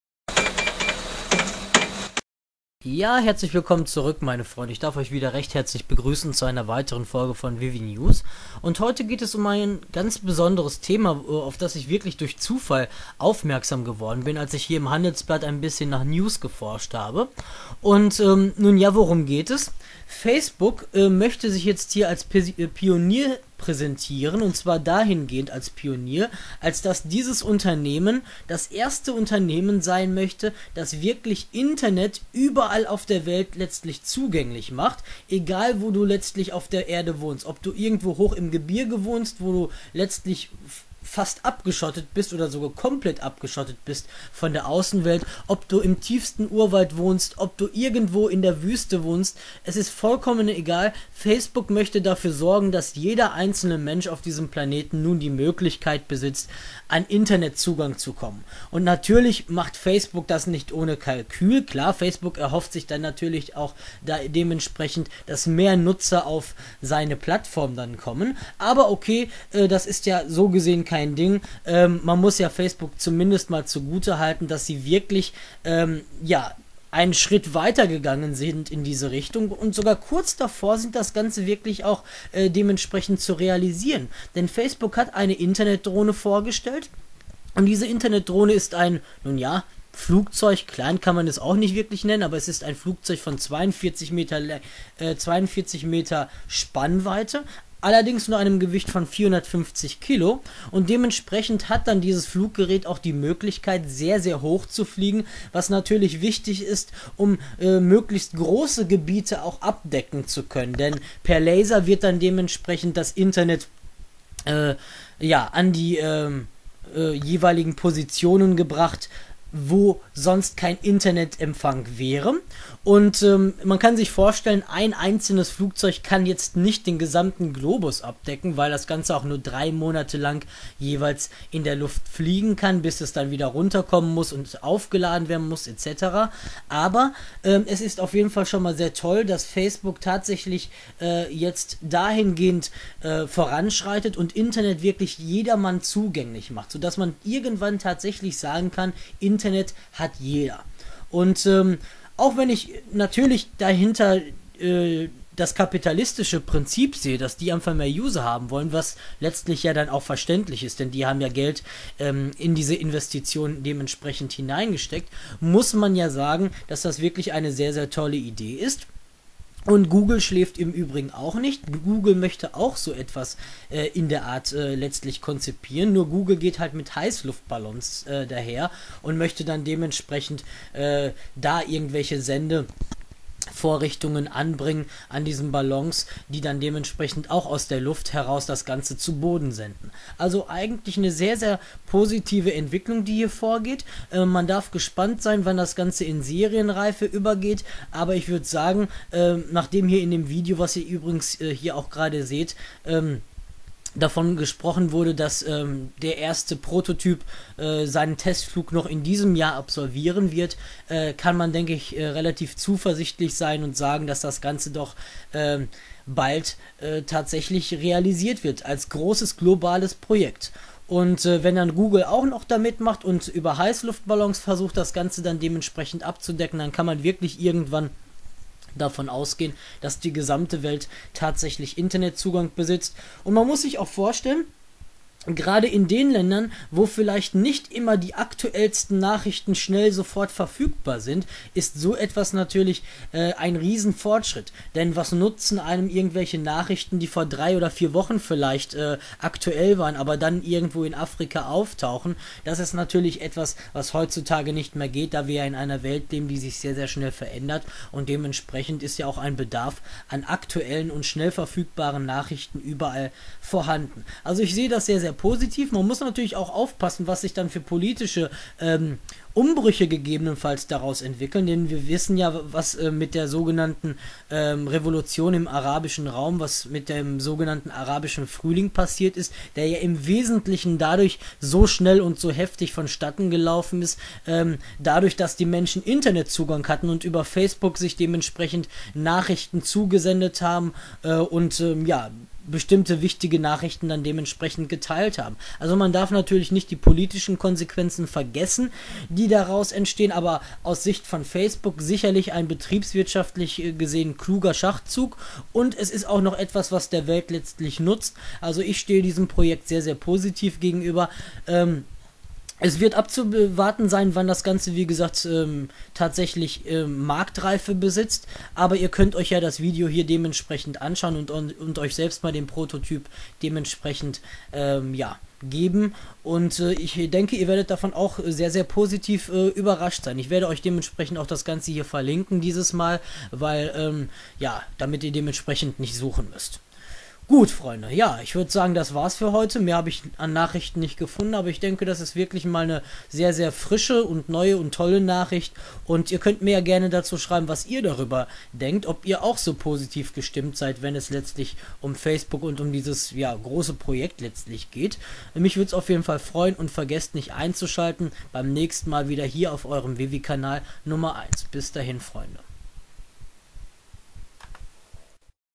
Facebook entwickelt Internet-Drohne
Facebook will sich als Pionier in der weltweiten Internetversorgung mithilfe einer Internet-Drohne versuchen, welche aus der Luft die entlegensten Orte mit Internet versorgen soll. Eine tolle Idee meiner Meinung nach.